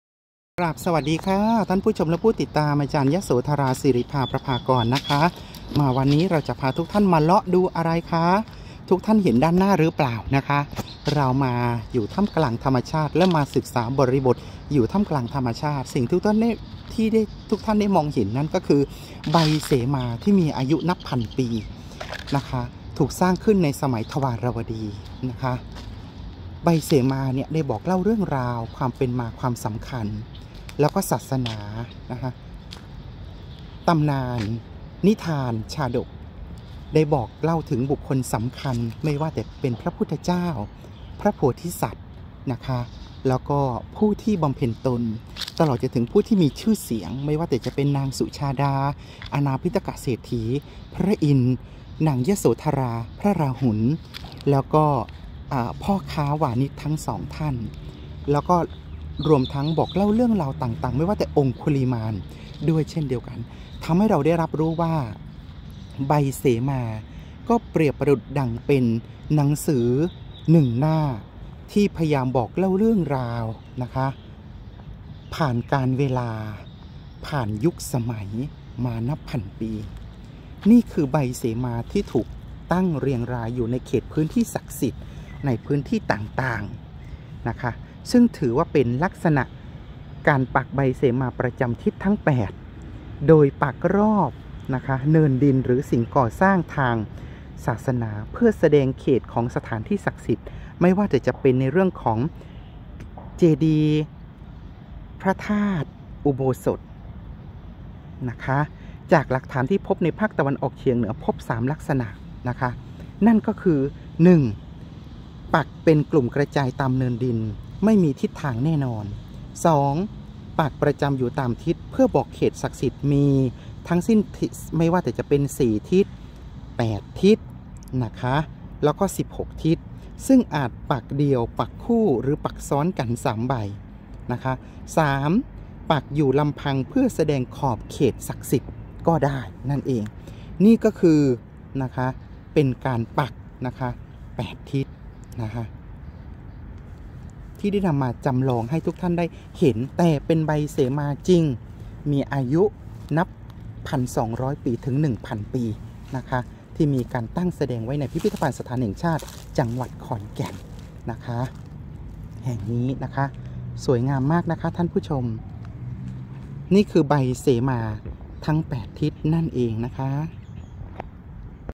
ลักษณะการปักใบเสมา 8 ทิศ
ศึกษาพุทธประวัติผ่านใบเสมาศิลาทราย สมัยทวารวดี อายุ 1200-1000 ที่ผ่านมา

สมัยประวัติศาสตร์และวัฒนธรรมทวารวดี ประชากรที่อาศัยในแถบที่ราบลุ่มแม่น้ำชีได้รับอิทธิพลวัฒนธรรมทวารวดี ซึ่งอาจผ่านมาทางเมืองศรีเทพ ปรากฏการสร้างศาสนสถานทางพระพุทธศาสนา โดยเฉพาะใบเสามาหิน เป็นต้น
ได้จากการลงพื้นที่ทำการศึกษา แผ่นศิลา ใบเสมา

วัตถุประสงค์เพื่อประโยชน์ทางการศึกษา และเชิญเข้าเที่ยวชม และทำการศึกษา ณ พิพิธภัณฑ์สถานแห่งชาติ ขอนแก่น
ผู้บรรยายผ่านวิดีโอ / อ.ดร.ยโสธารา  ศิริภาประภากร  (นักวิชาการอิสระด้านพระพุทธศาสนาและวัฒนธรรมท้องถิ่น กับสังคม)
Dvaravati history and culture Populations living in the plains of the Chi River were influenced by the Dvaravati culture. which may pass through the city of Si Thep It appears that the construction of religious places of Buddhism Especially the Sao Ma Hin leaves, etc.
Acquired from the study area, Sela Baisema stone plates

Objectives for educational benefits and invited to visit and conducted a study at the National Museum, Khon Kaen
Video speaker / Dr. Yasothara Siriphaprapagon (Independent scholar on Buddhism and local culture and society)